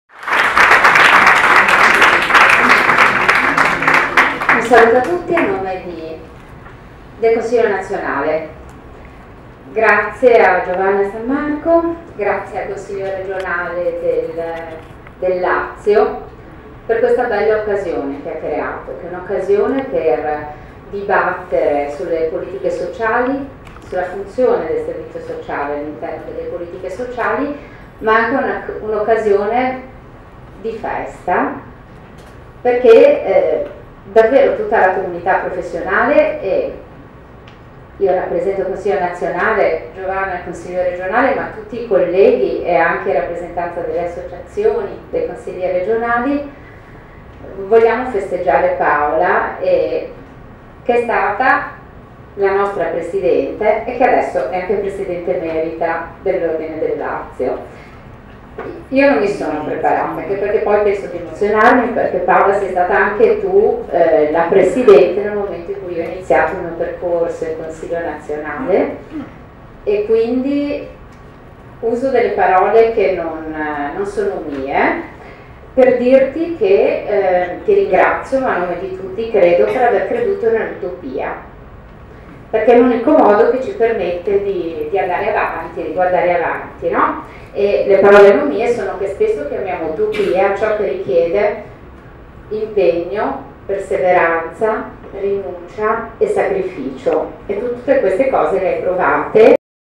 Silvana Mordeglia, Presidente Nazionale Ordine Assistenti Sociali
Il consiglio Regionale dell'Ordine Assistenti Sociali del Lazio, d'intesa con il Consiglio Nazionale, ha deliberato il conferimento dell'onoreficienza di "Presidente Emerito" all'assistente sociale specialista Paola Rossi  (primo presidente dell'Ordine Nazionale degli Assistenti Sociali), per l' impegno costante, la passione con la quale ha svolto la professione e per l'impulso fondamentale dato dall'istituzione dell'Ordine; la consegna dell'onorificenza  ha avuto luogo il 14 Maggio 2015 durante il dibattito sul tema: Welfare oggi? Il ruolo del servizio sociale" presso la Sala Riunioni Via della Greca 5, Roma.